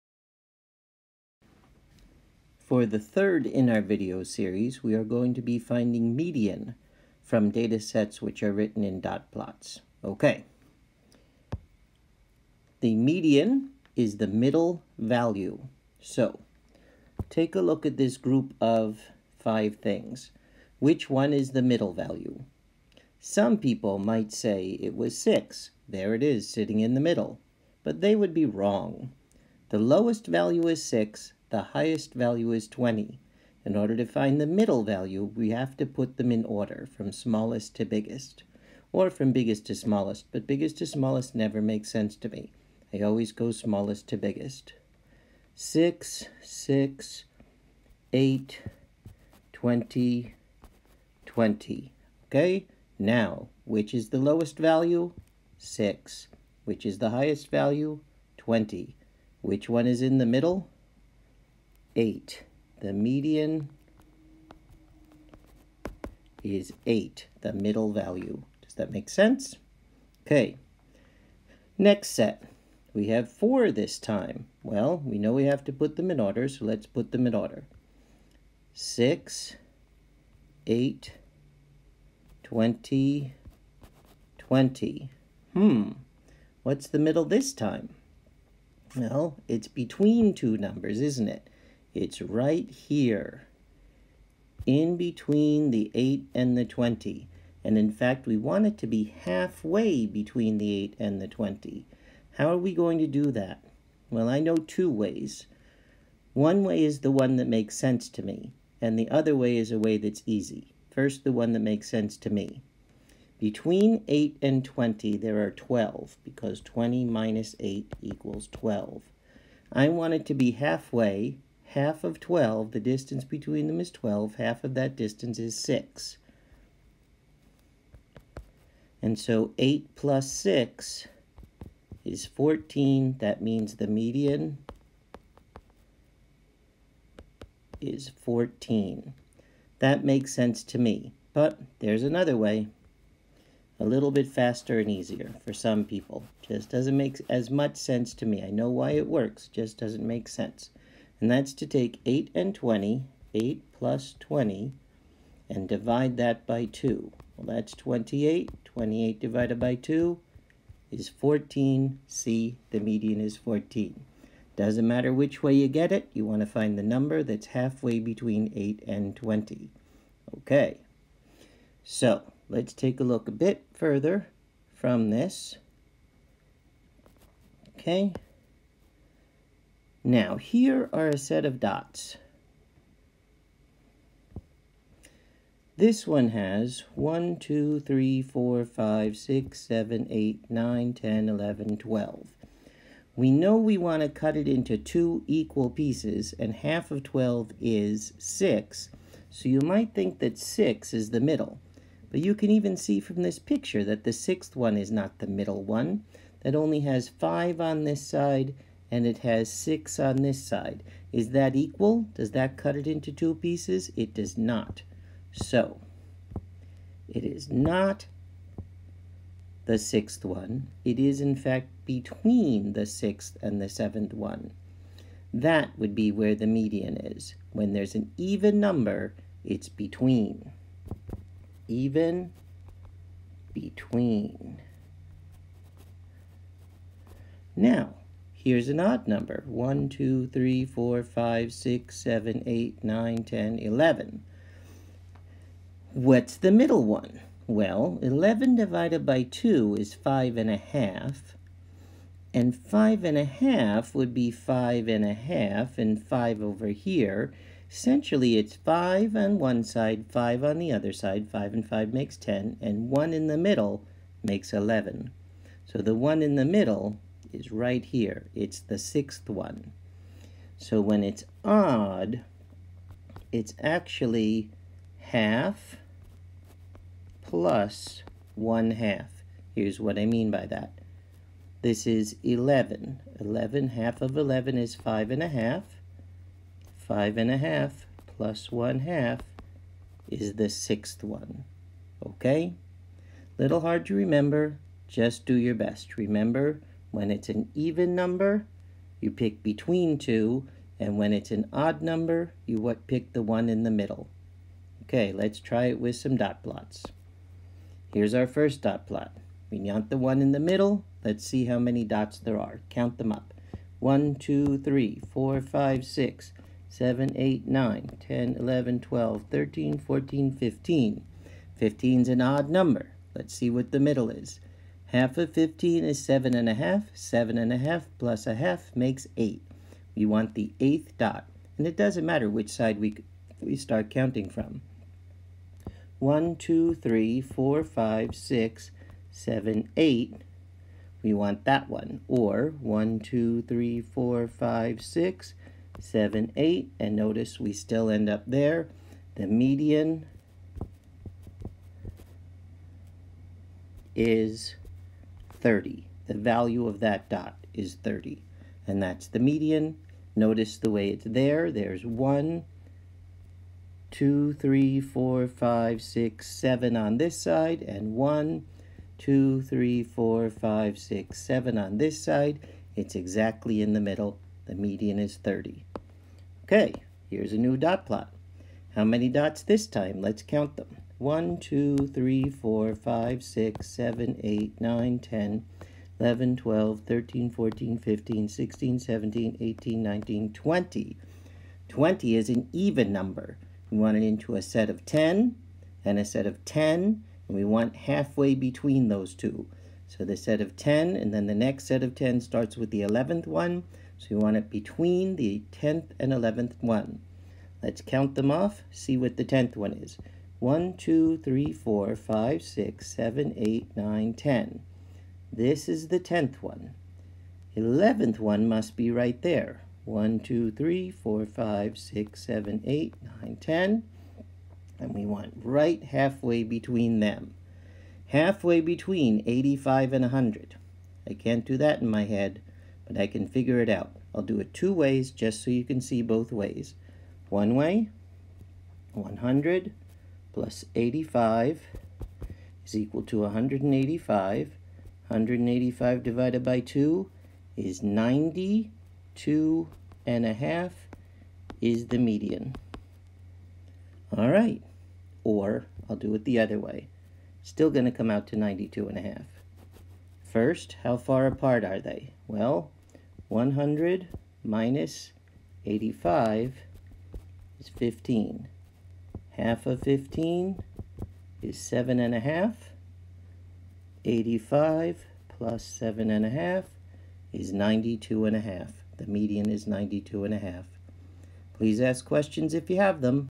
Finding Median from a Dot Plot
How to find the median for a set of data that is organized in a dot plot